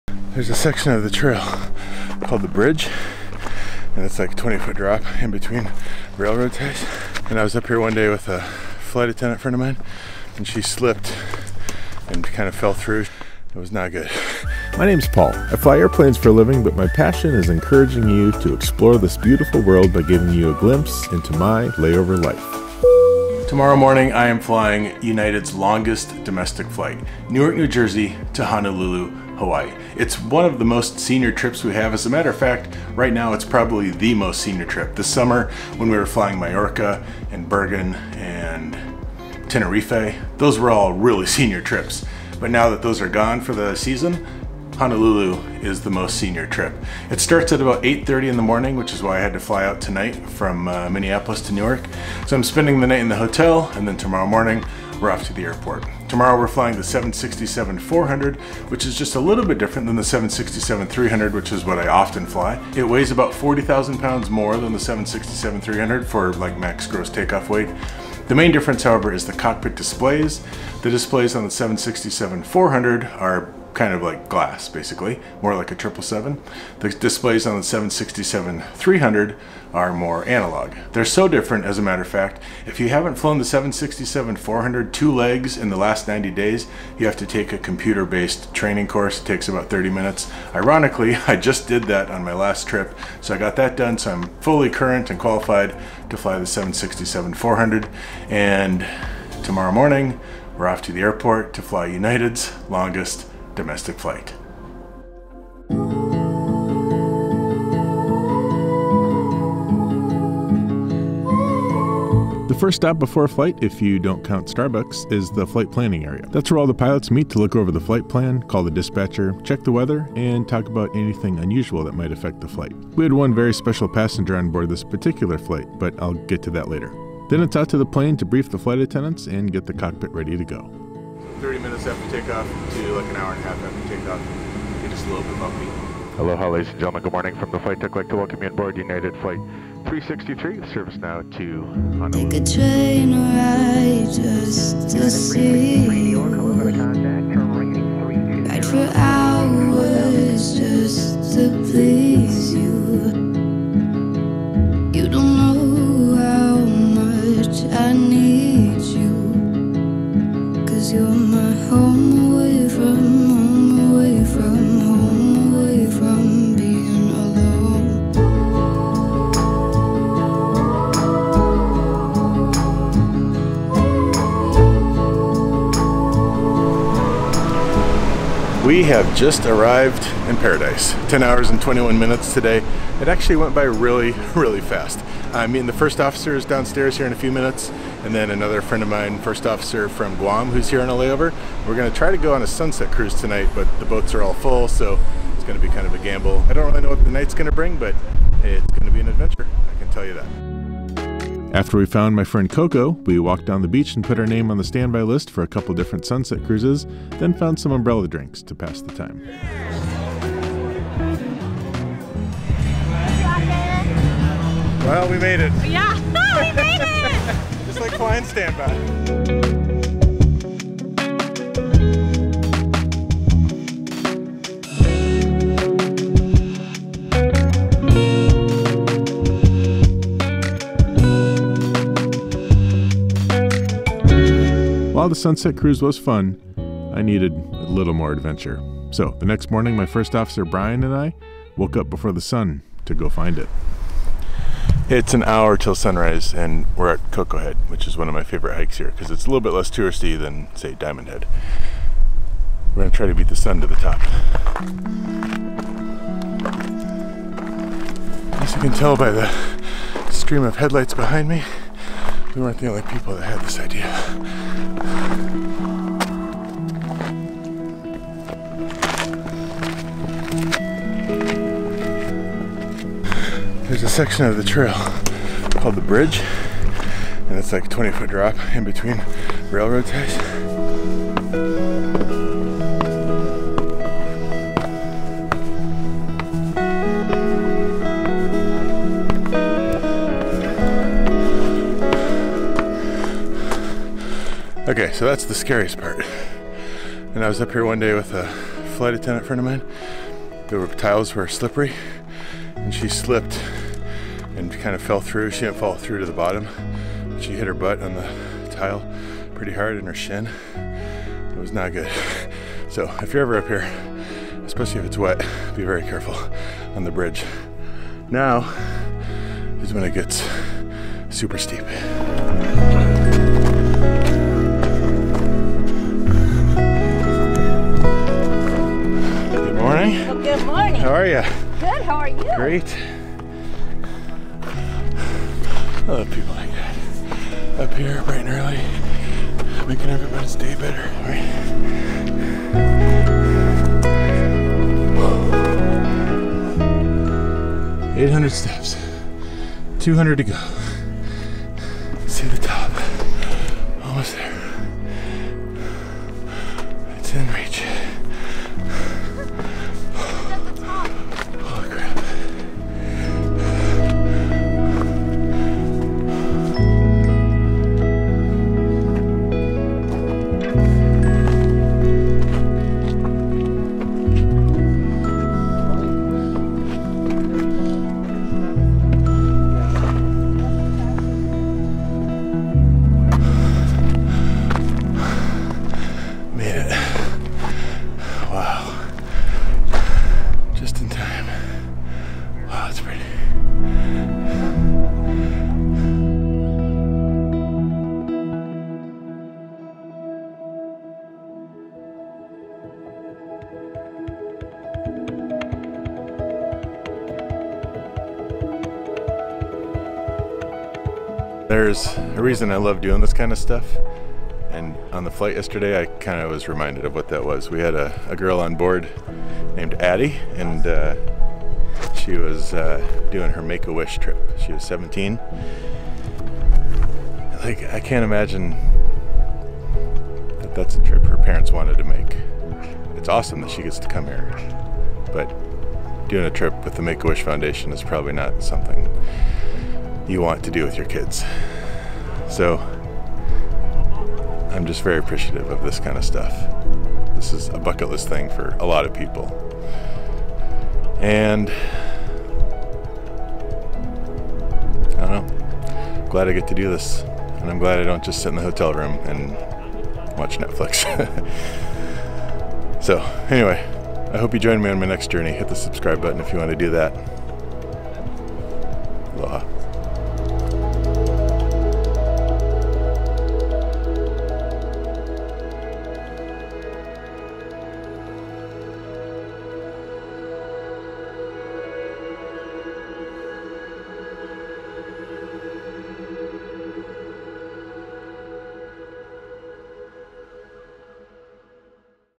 WORKING UNITED'S LONGEST DOMESTIC FLIGHT
Join me as I work United's longest domestic flight! We might even find a few adventures to go on.

My Gear:
Vlog Camera: Sony ZV-1
Microphone: Rode VideoMicro
Drone: DJI Mini 3 Pro

00:00 Intro
00:22 Our Most Senior Trip
00:56 Cockpit Differences
01:46 Preflight
02:23 Newark to Honolulu
02:58 Debrief
03:20 Waikiki Adventure
04:03 Sunrise Hike
06:50 Why I Seek Adventure